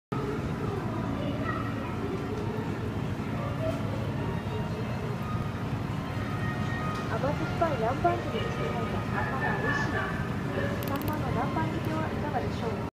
鳥取松葉ガニと親ガニの生簀販売
鳥取松葉ガニ生簀販売。特に箱に入れてもらえるのではなくそのまま持ち帰りです♪